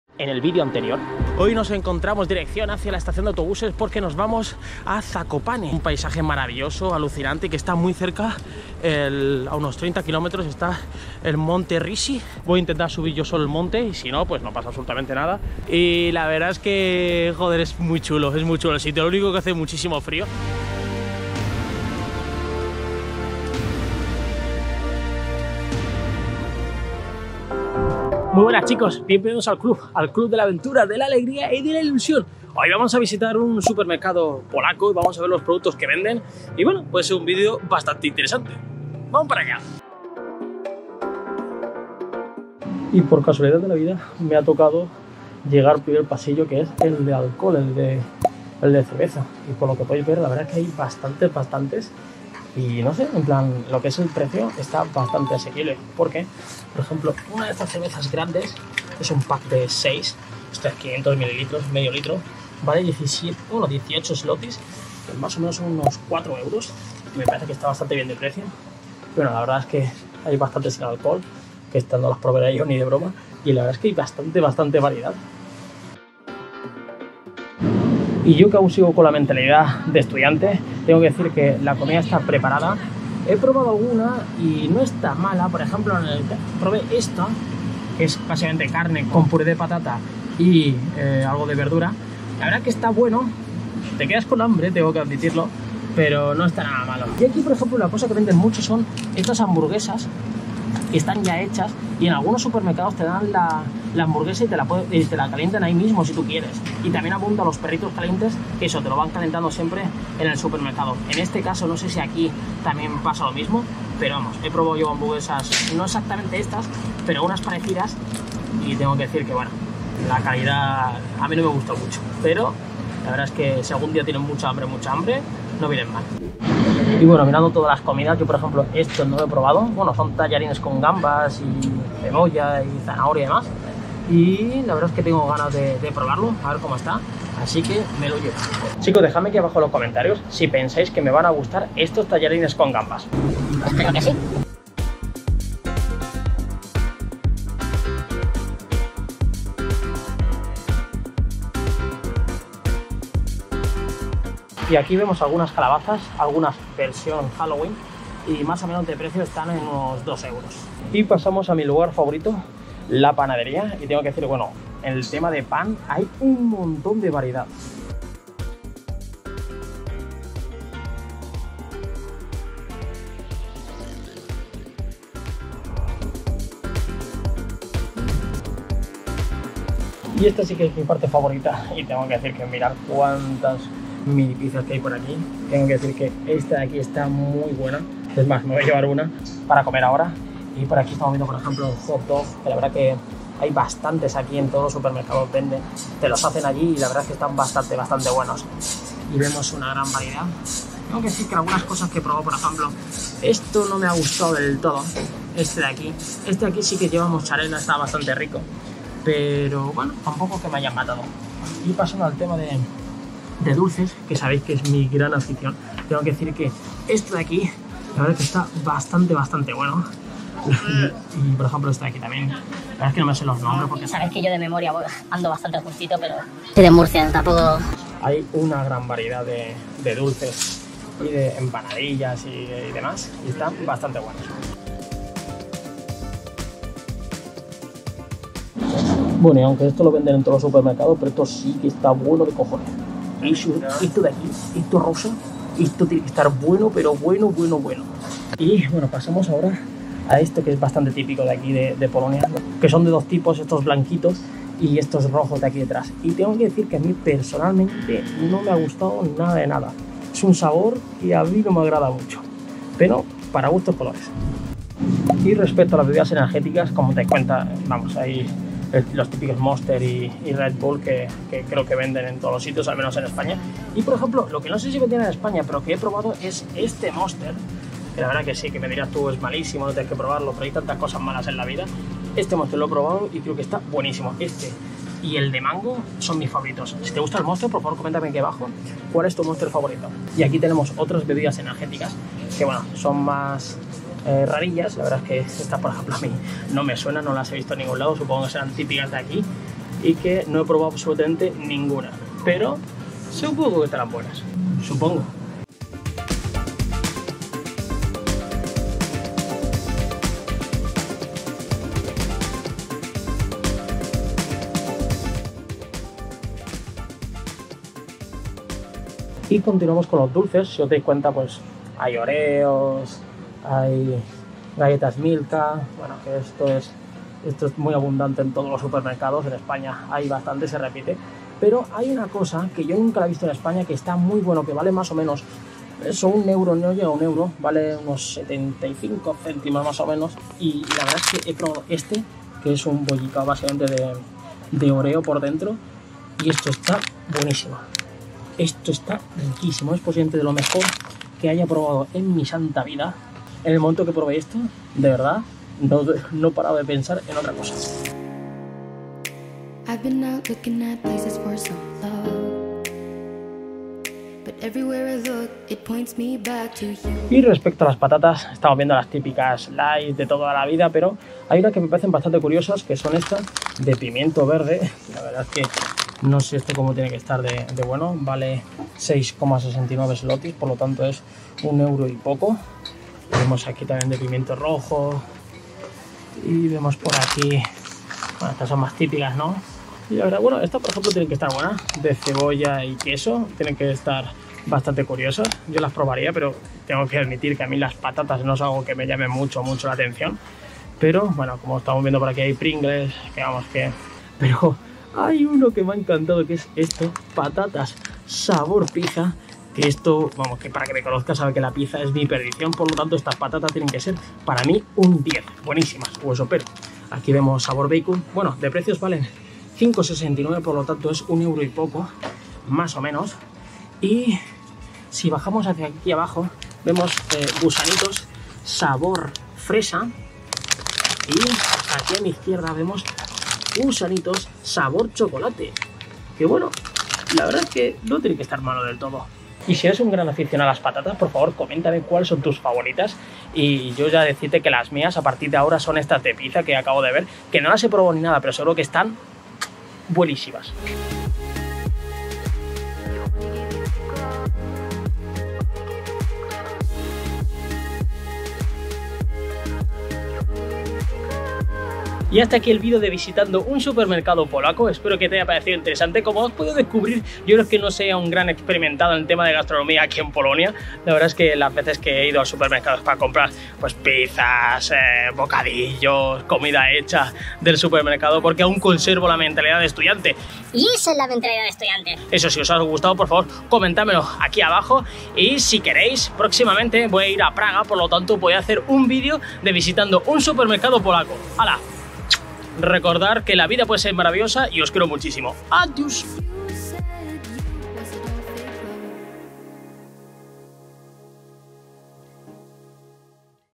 ¡SUPERMERCADO POLACO! 🇵🇱 ¿Comprar en Polonia es barato? | Viajando por Europa sin rumbo
En este vídeo os muestro un supermercado Polaco/Polaco viajando sin rumbo por Europa, qué productos podemos encontrar y cuáles son sus precios, ¿comprar en Polonia es barato o caro?, por un problema con la tarjeta de memoria se borraron varios clips :(

📺 Mira mis videos anteriores 📺

🎬¿Cómo es Varsovia? Subo al EDIFICIO más ALTO del PAIS | POLONIA | Viajar con poco dinero y barato